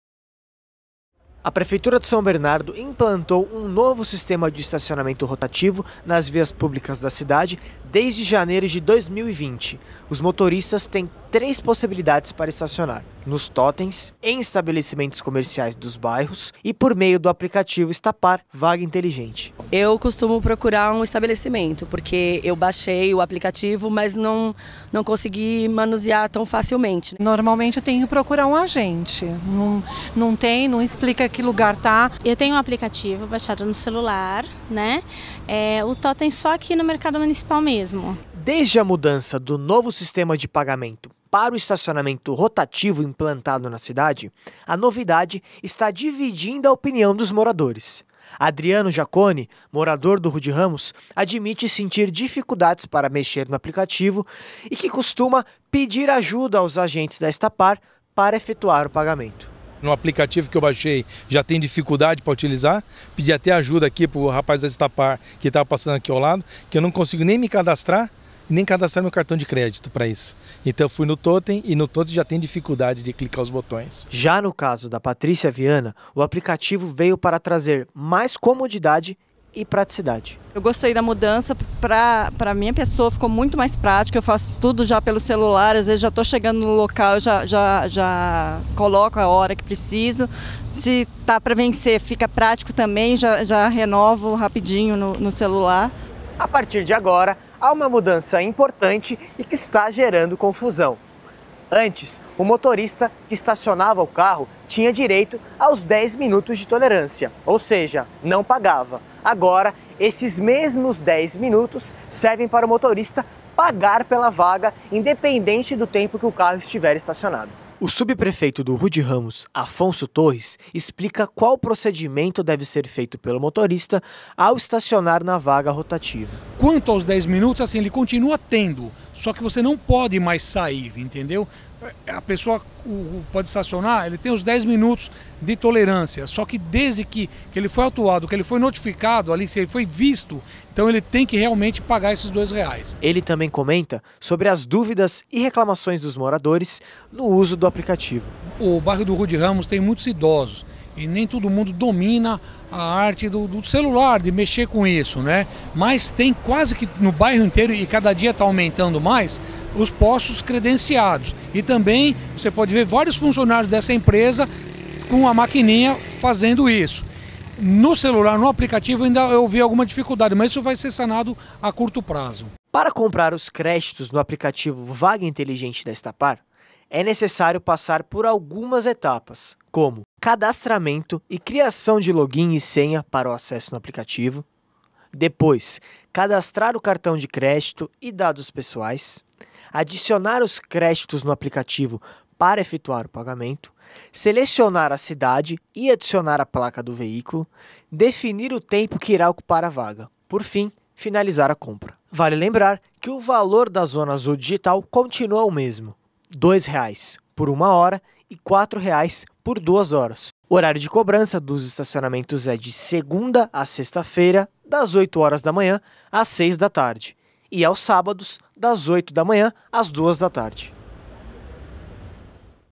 Estacionamento Rotativo no Rudge Ramos
A nova maneira para estacionar carros no bairro do Rudge Ramos vem gerando dúvidas para os moradores.